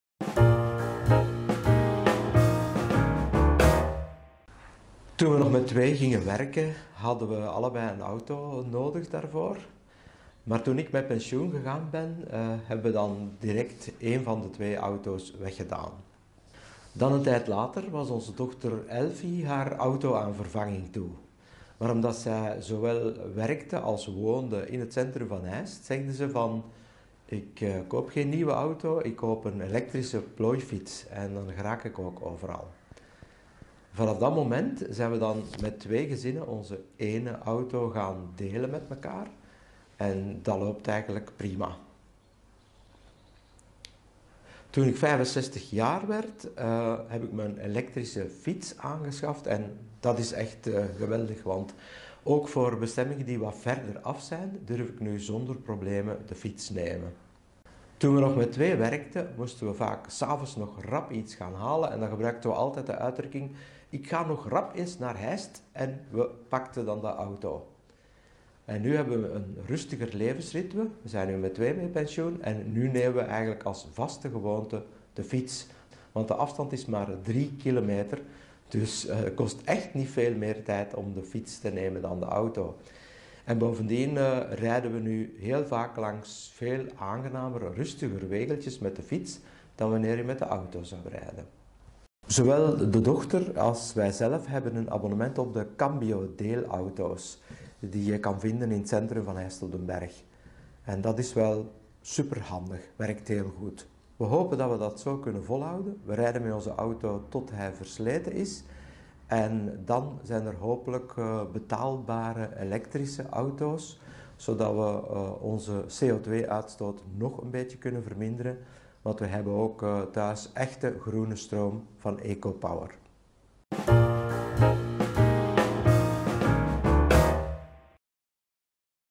Dirk en Elfi - een auto minder
Dirk Ceulemans vertelt voor 'Duurzame Heistenaars' over leven met een auto minder.